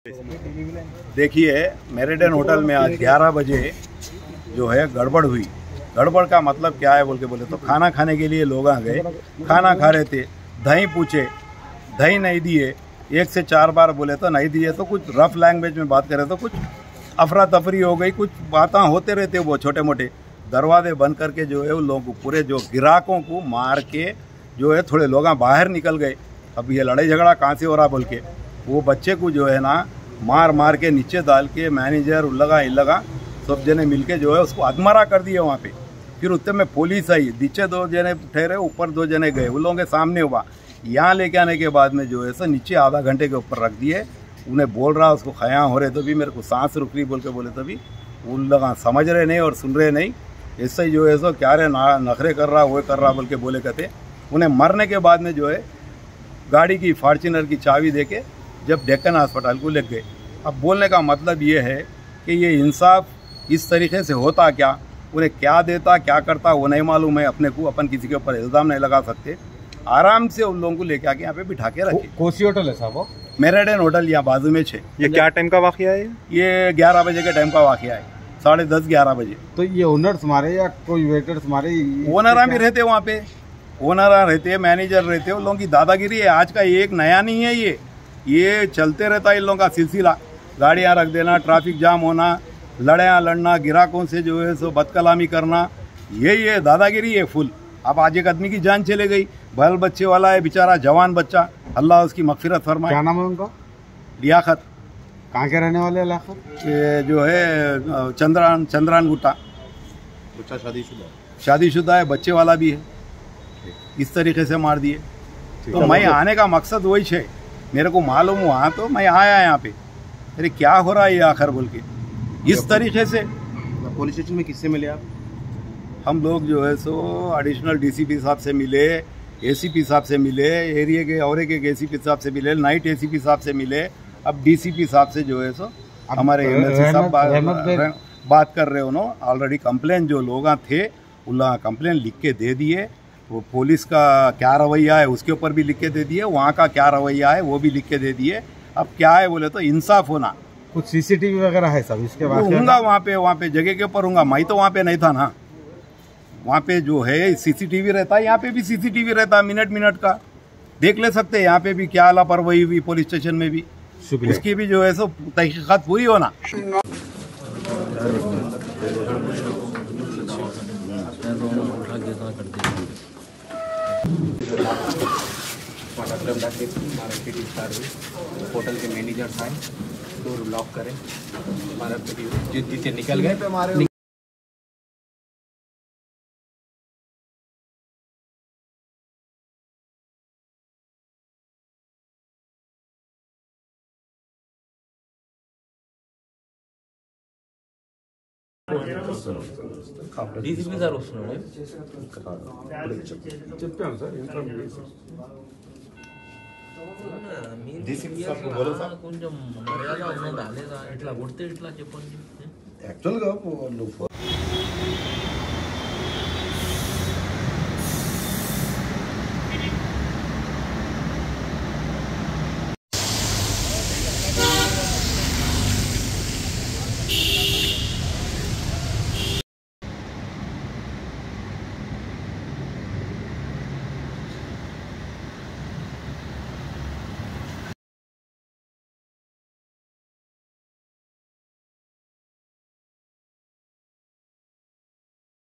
*breaking#MURDER_Panjagutta per Maujuda "Meridian_Hotel walon ki Gundagardi ek Customer ko Hotel k..
*breaking
MURDER_Panjagutta per Maujuda "Meridian_Hotel walon ki Gundagardi ek Customer ko Hotel ke Owner aur Waiters milke kardiya Qatal...

Ibtedai Itelat ke Mutabiq ek customer se kuch kharab khaney per hua Jhagde mein Hotel ke Owner aur Waiters ne milke kardiya hamla jisme Customer ki Spot per he M@ut Hogayi, Itela Milte he Panjagutta police aur MLC Rehmat baig Pahonche Spot per...
Sabeqa mein bhi Meridian hotel mein kai bar Customers per hamla karne waqiyat pesh aaye hai...